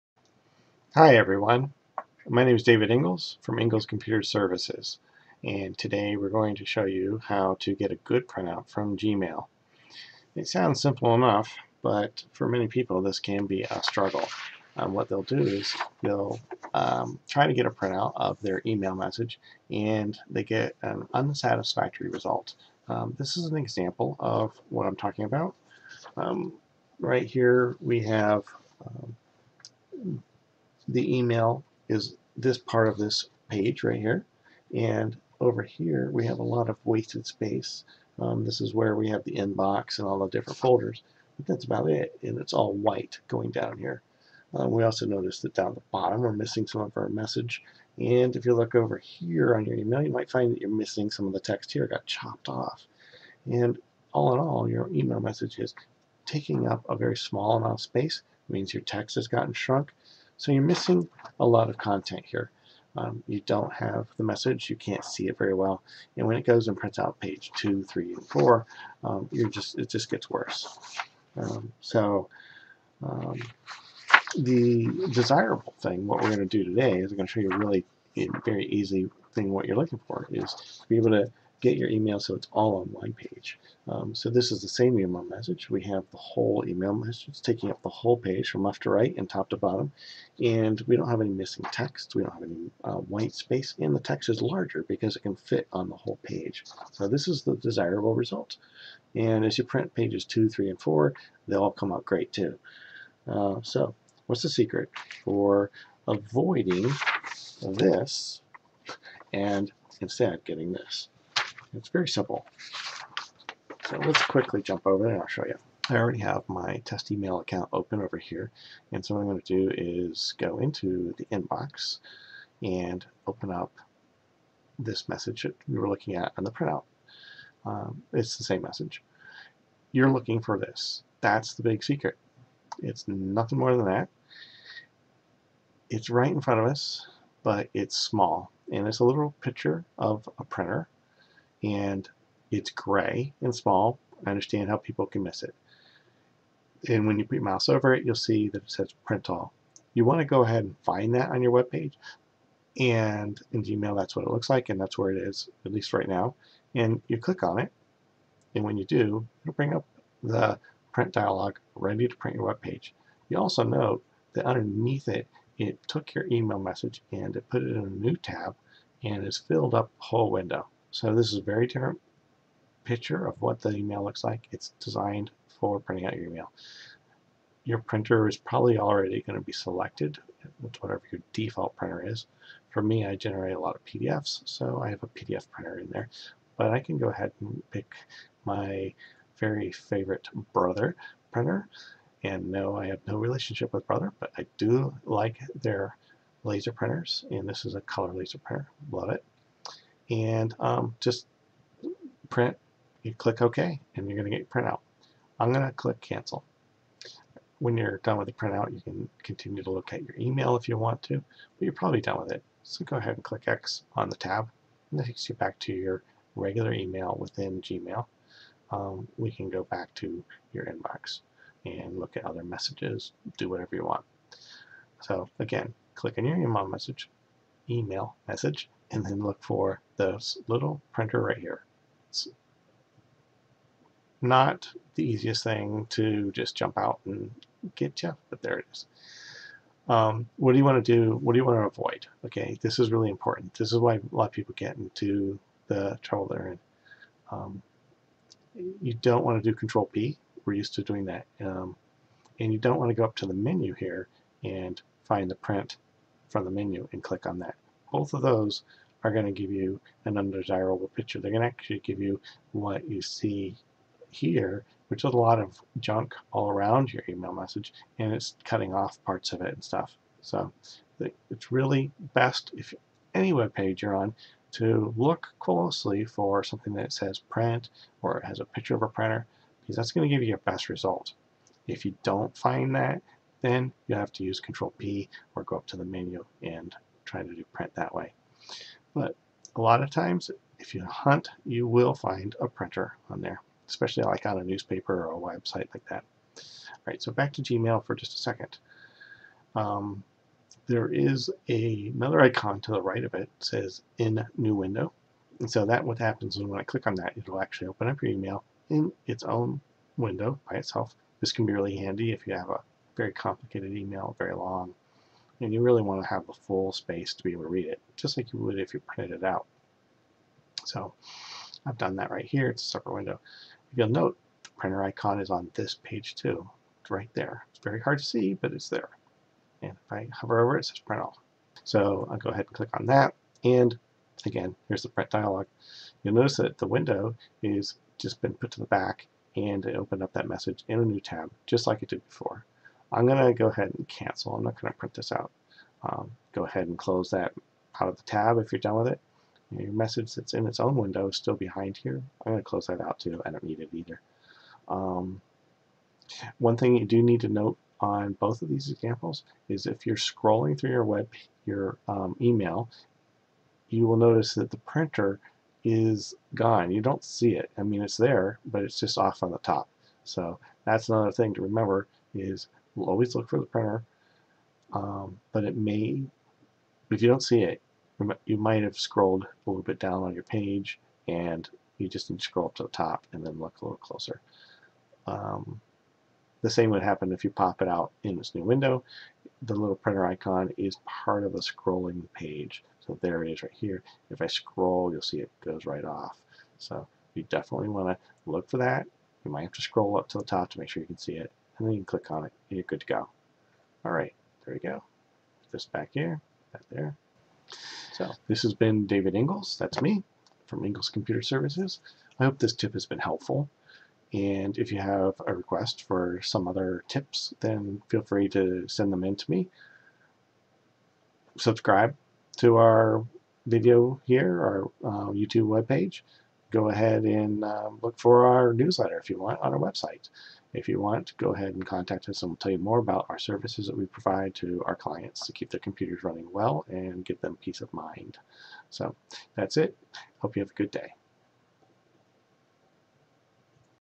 Getting Great, full size, Printouts from Gmail Email
Why doesn't my Gmail printout look right? Is it too small? Hard to read? Missing parts of the email message? Watch this video to learn how to best print out your Gmail email messages full page!